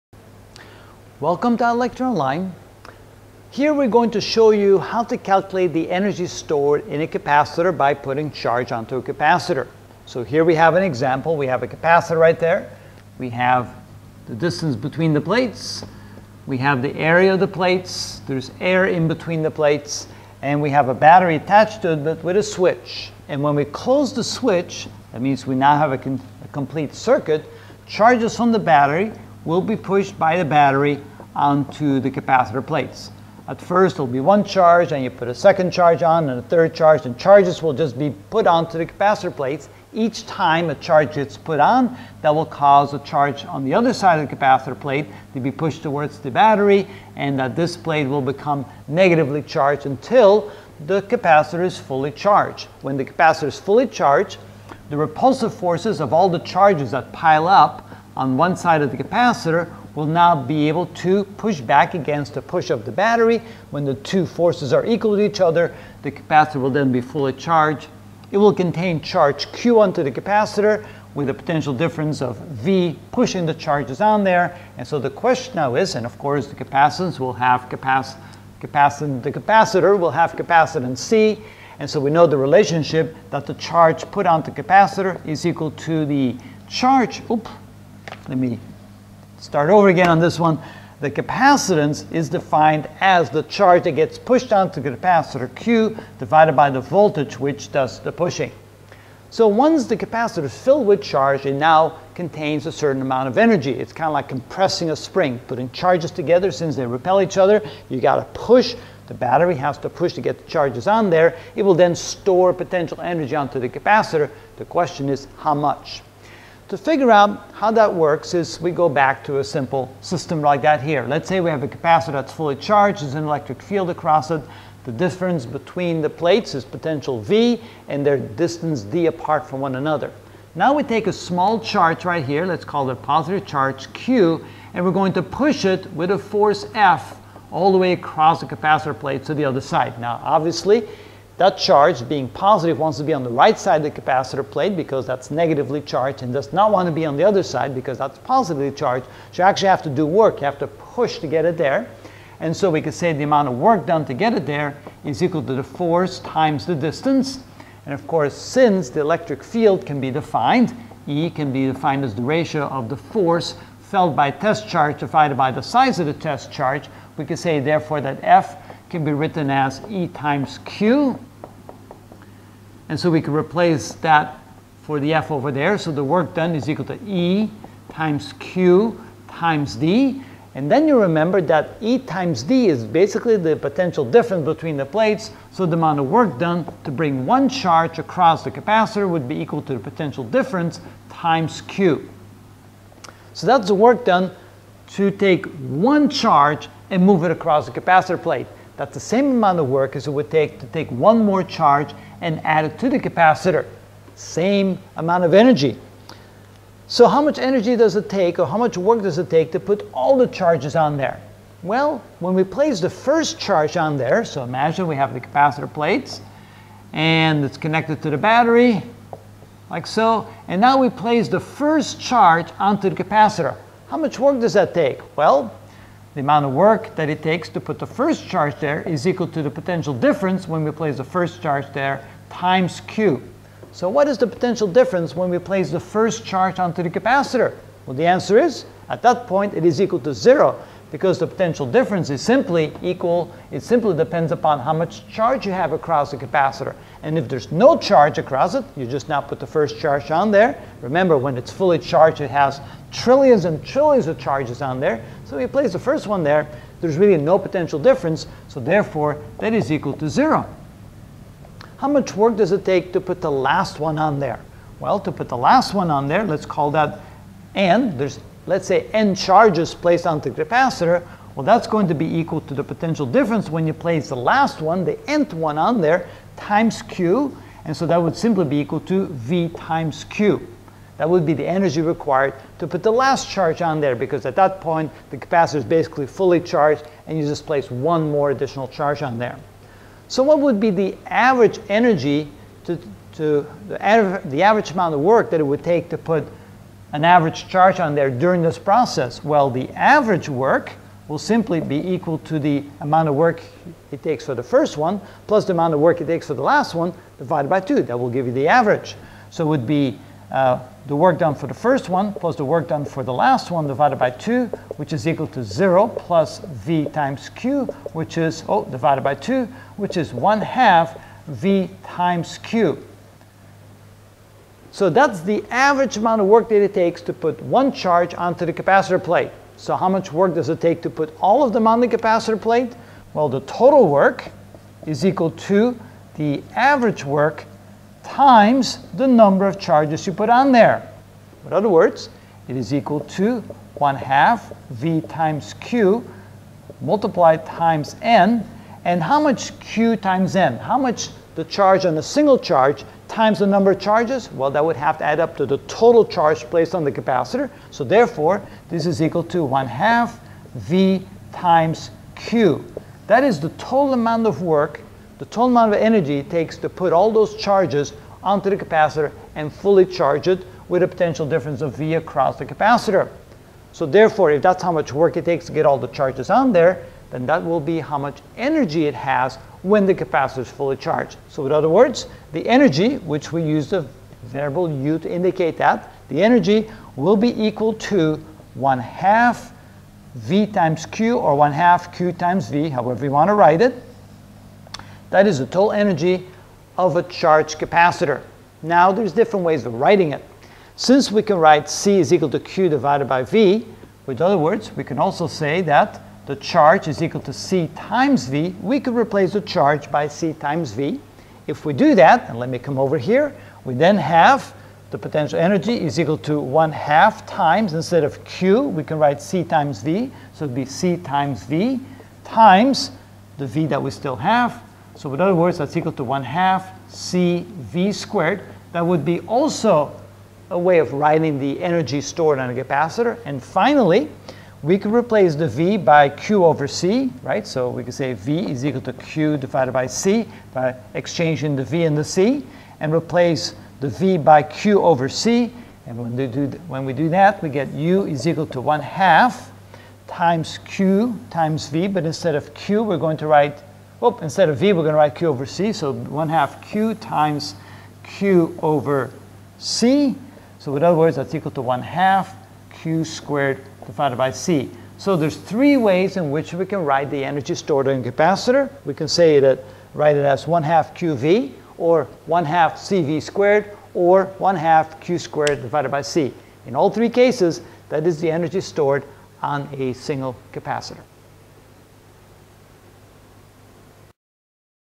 Physics 39 Capacitors (15 of 37) Energy Stored on a Charged Capacitor
In this video I will calculate the energy stored by putting a charge onto a capacitor.